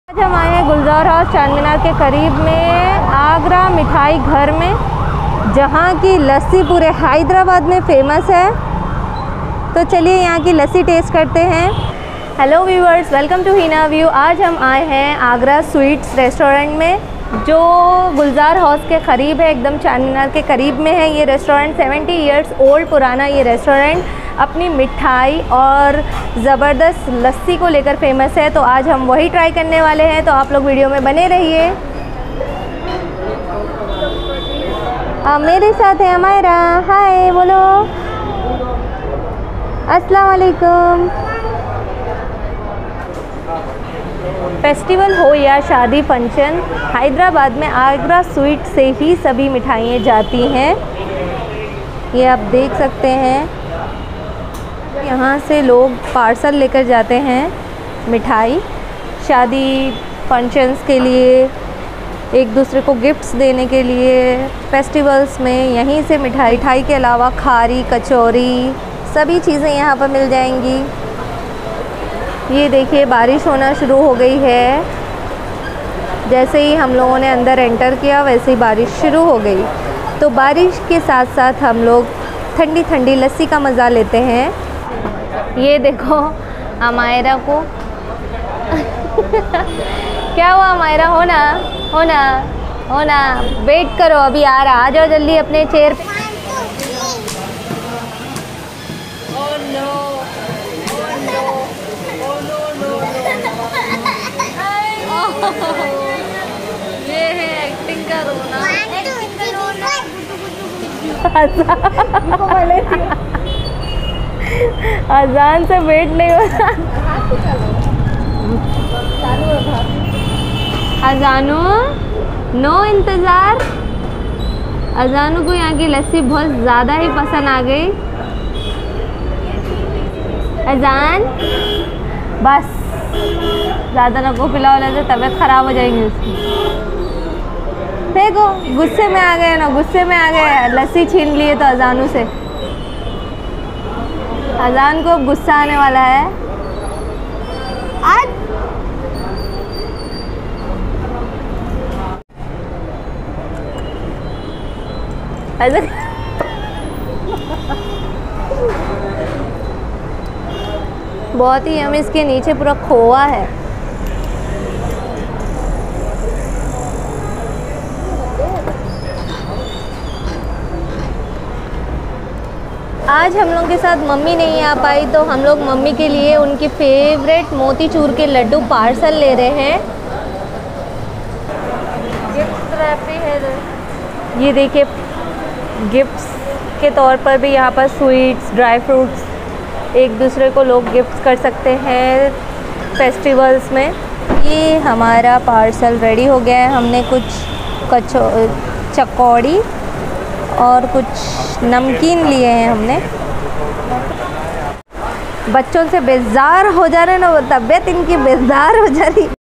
AGRA MITHAI GHAR | Charminar No 1 Sweet Shop 70 Years Old
Hello friends welcome to heenaview

 today I visit to Agara mithai Ghar 70Year Old Resturant

Agra sweet house best sweets and the purity they maintain was excellent and specially lassi and hot kesar badam milk was awesome and the oldest

My one more
channel

Moj..

Mxt takatak.

Josh.